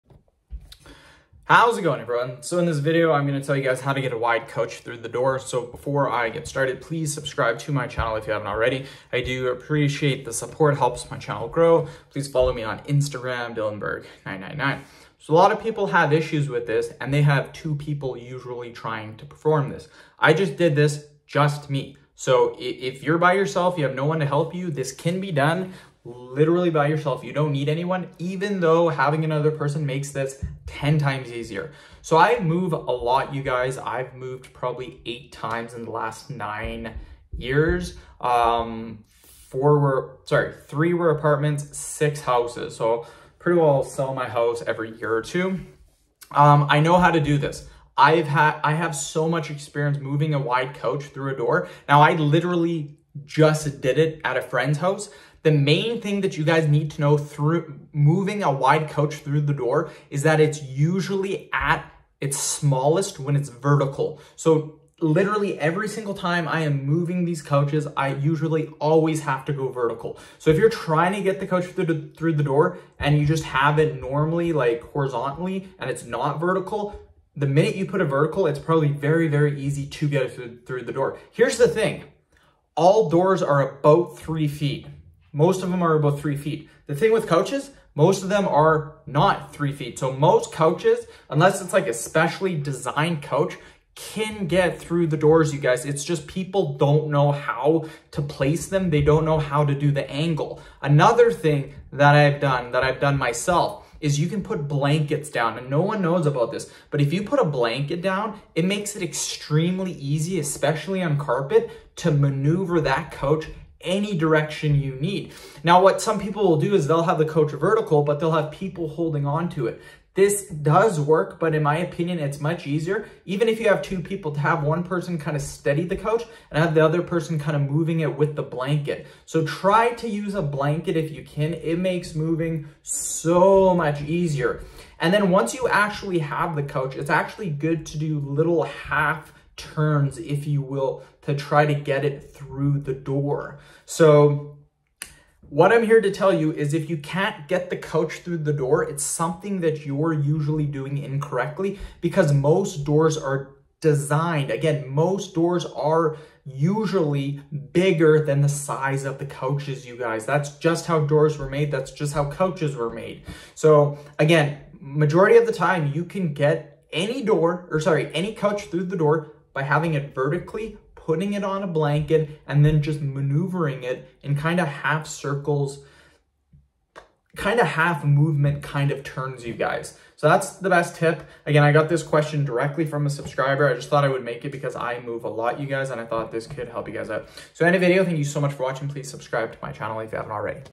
how to get a wide couch through the door
how to get a wide couch through the door. Here in this video i talk about how to get a wide couch through the door.